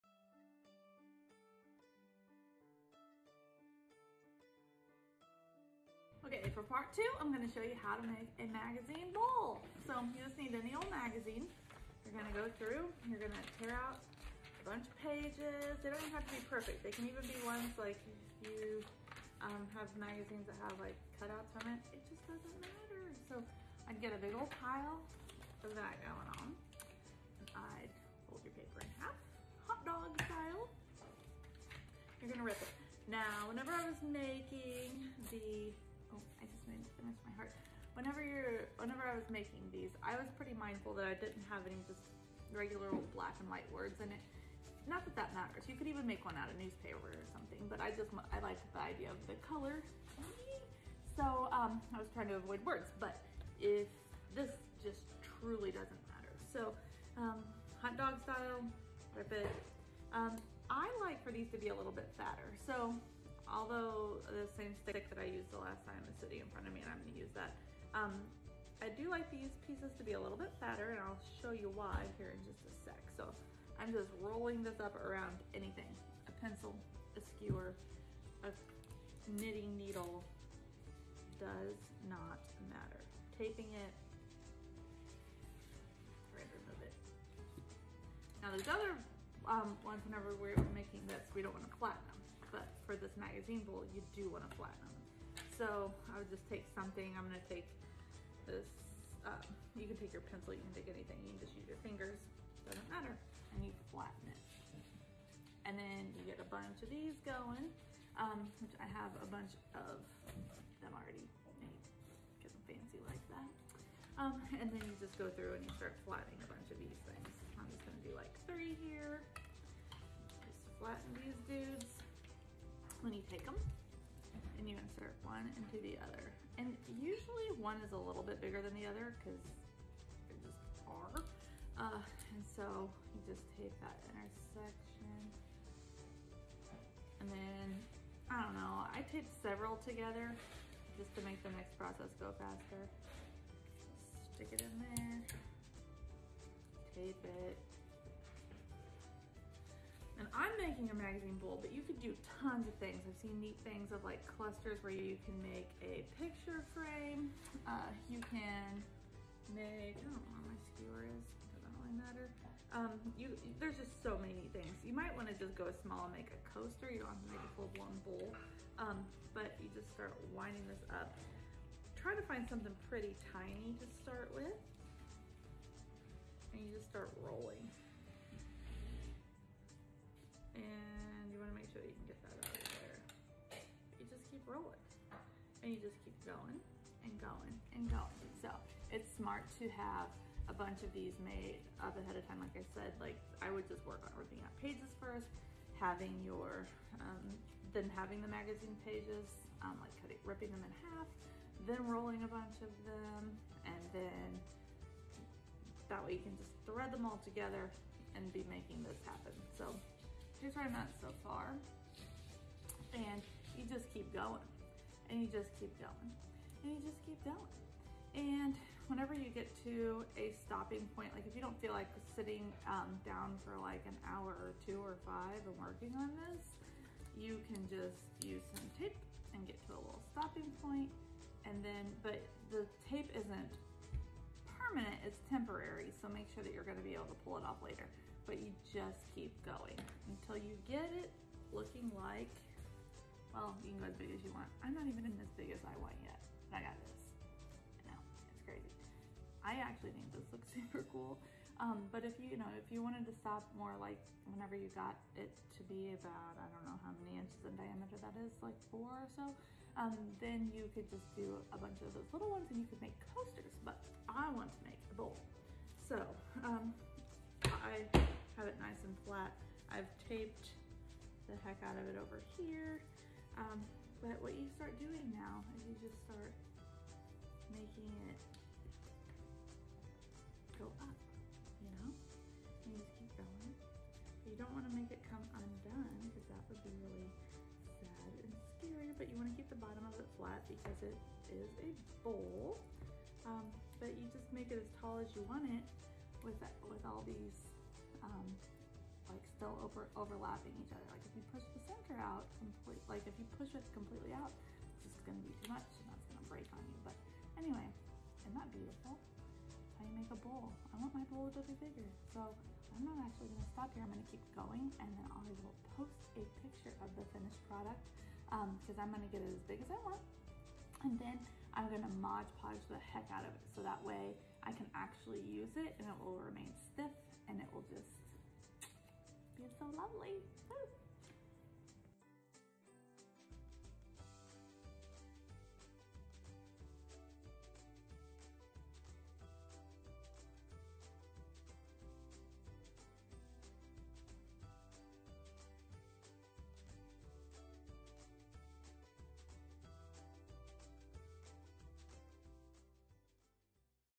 Magazine Art - Part II - Fancy Bowl
You will need:

magazines
tape
a pencil or something to roll your magazines
Modge Podge - to seal your product to keep it secure! You don't need this immediately!

Time consuming but worth it! If you don't have a ton of patience, start by attempting to make a coaster or tiny bowl!